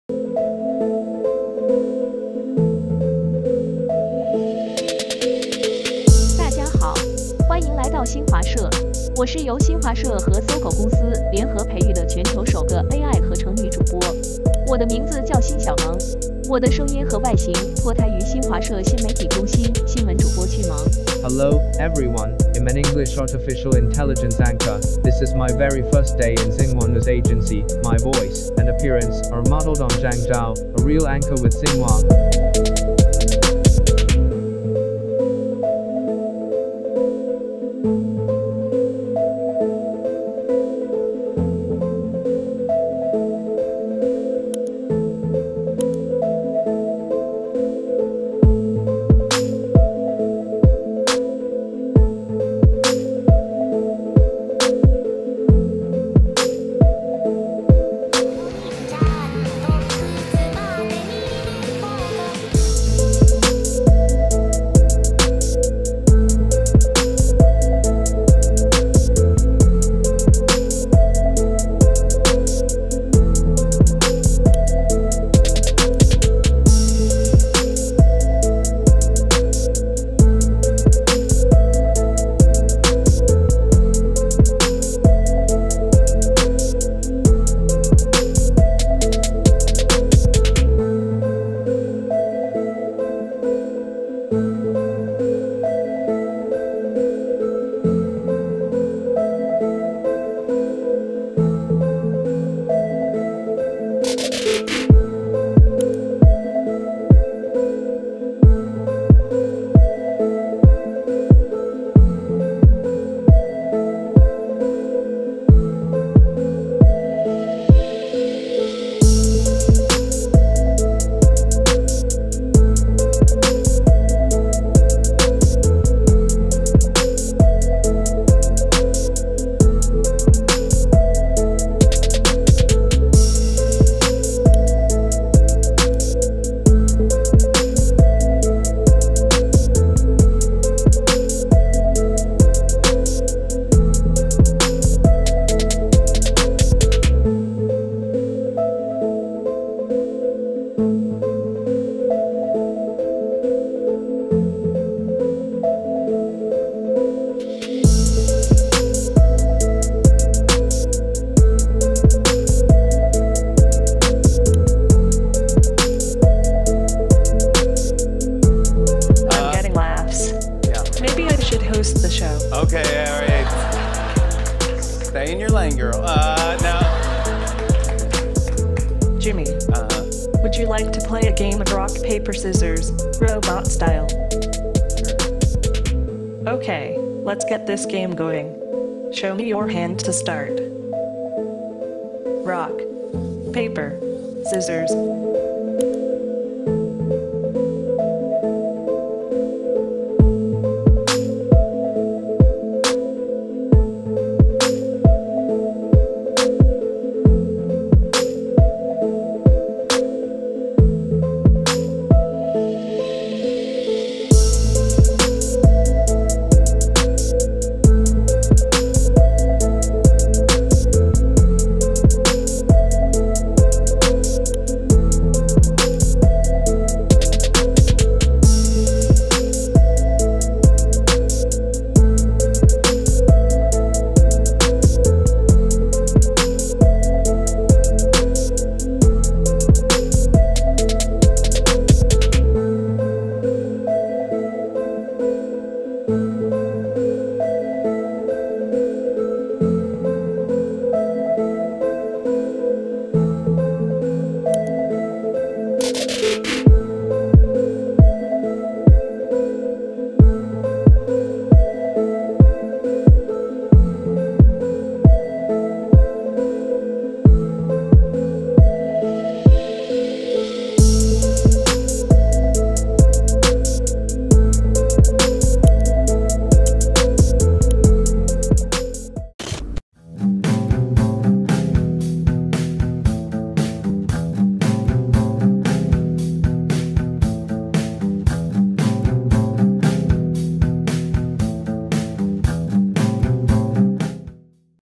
Robot Dah Lama Rancang, Kita Yang Lalai: 10 Perkembangan Robot 2022
Memang advance gila teknologi robot sekarang sampai boleh buat benda² ini.